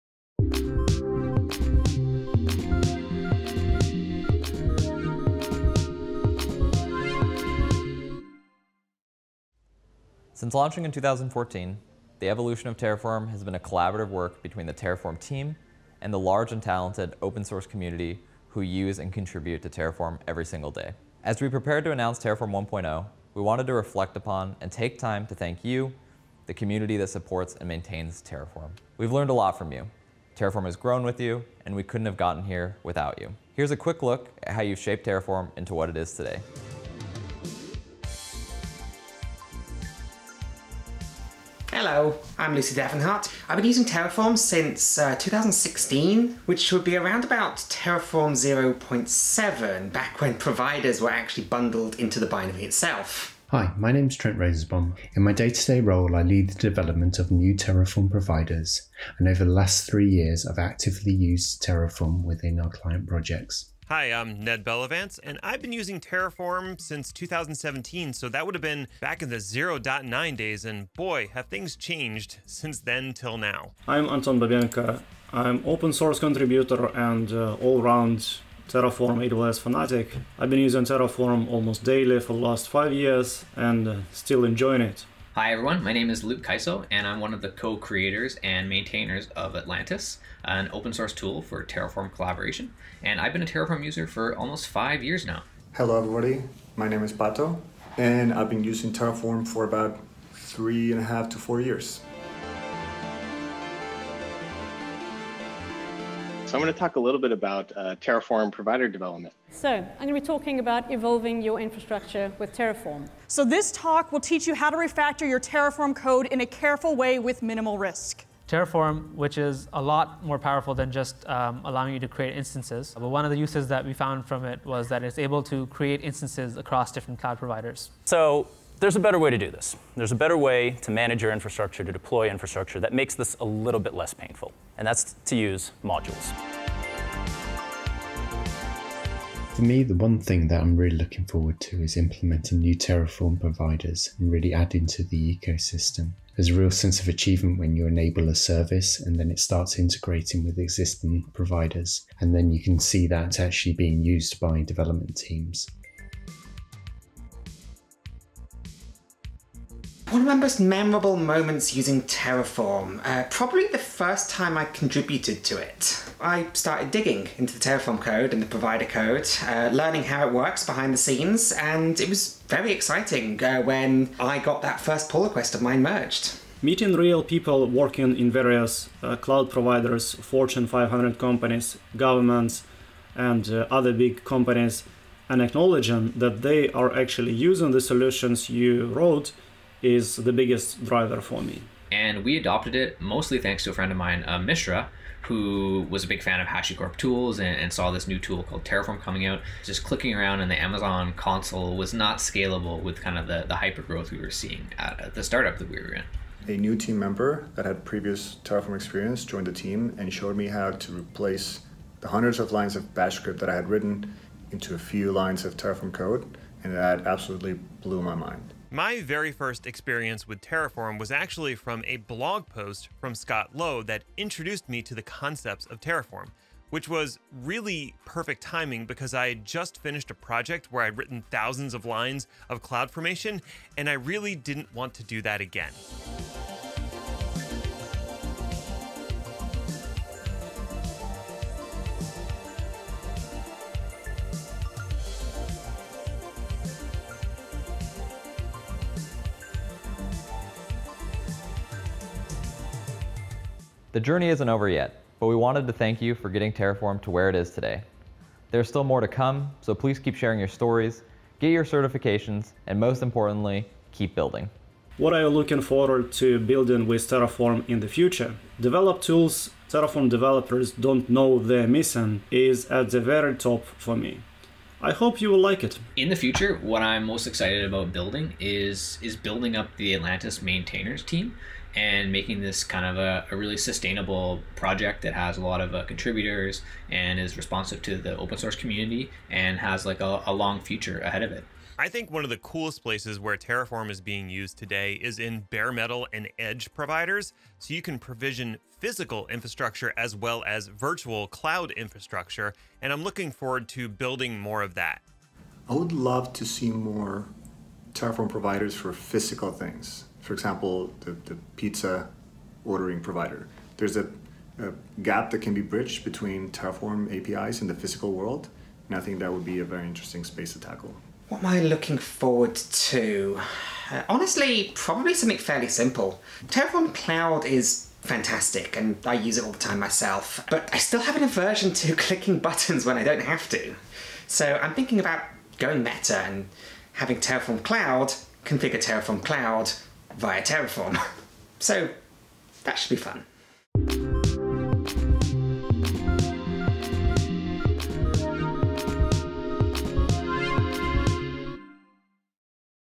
Thank You to the Terraform Open Source Community
Since its launch in 2014, Terraform has been a collaborative work between HashiCorp and you, the open source community that uses it every day. We couldn't have gotten here without you, so we wanted to say thanks.

In this video, you will hear from Mitchell Hashimoto, Co-Founder and CTO of HashiCorp, Luke Kysow, Co-Creator of Atlantis, and HashiCorp Ambassadors Lucy Davinhart, Anton Babenko, Ned Bellevance, Trent Rosenbaum, and Pato Arvizu.